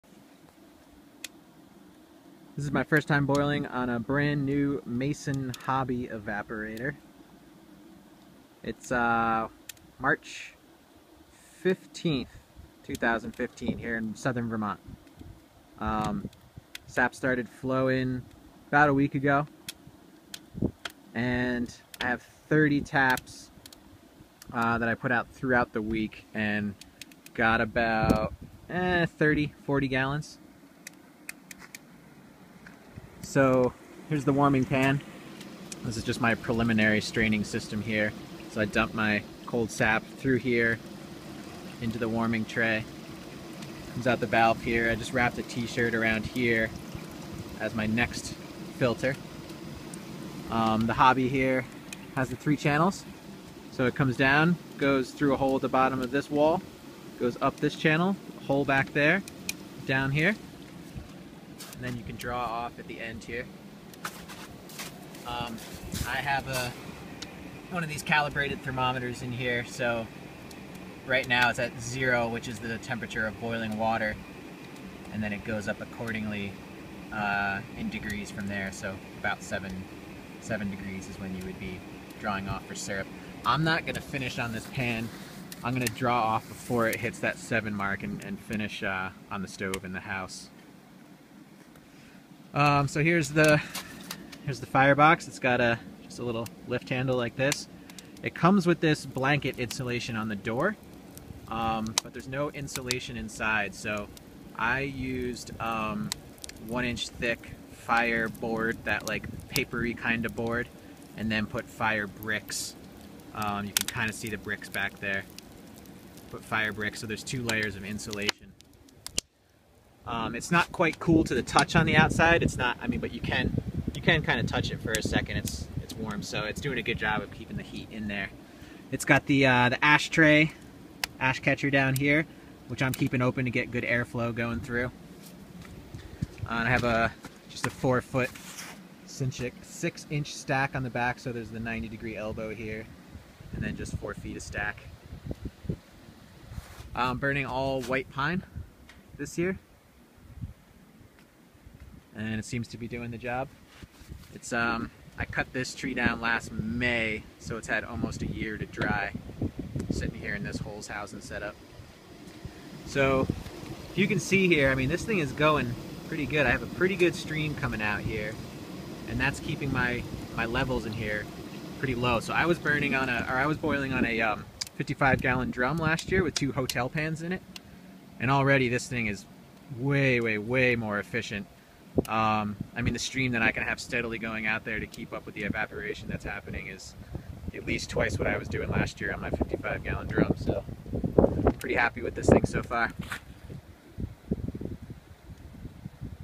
Mason Hobby Evaporator first boil. Small maple arch and pan for weekend warriors.
After using a homemade barrel evaporator last year for my maple syrup making, I upgraded to a W.F. Mason Hobby evaporator. So far I am extremely pleased. Sooo much more efficient than my old evaporator.  It almost feels like cheating. This is only my first time using it -- we tapped 30 trees this week here in Southern Vermont and the saps been flowing pretty well, got about 30 gallons this week.